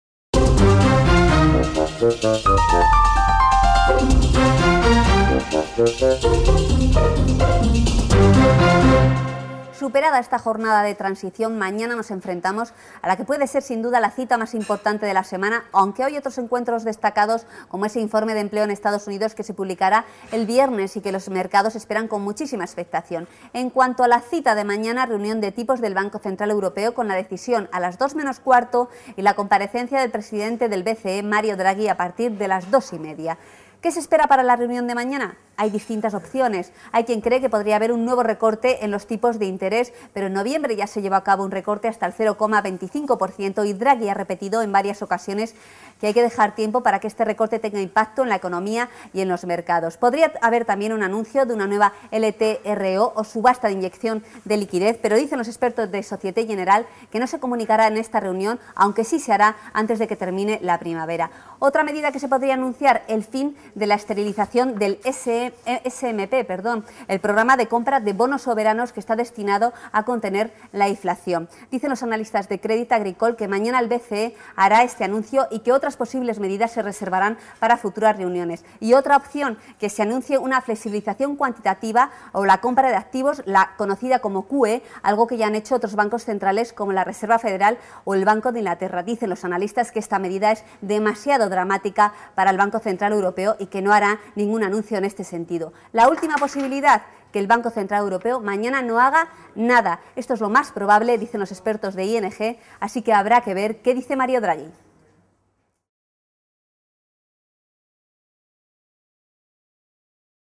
Radiografía de la sesión: Y ahora, todos pendientes de lo que diga mañana Draghi
Tras una jornada de transición, llega mañana la que será la cita más importante de la semana (con permiso del Informe de Empleo de Estados Unidos del viernes). Mario Draghi, presidente del BCE, hablará el jueves a partir de las 14:30 horas y desvelará si finalmente el organismo cuenta con la información suficiente, como dijo en febrero, como para tomar alguna medida excepcional. La más probable, el fin de la esterilización del SMP (programa de compra de bonos soberanos).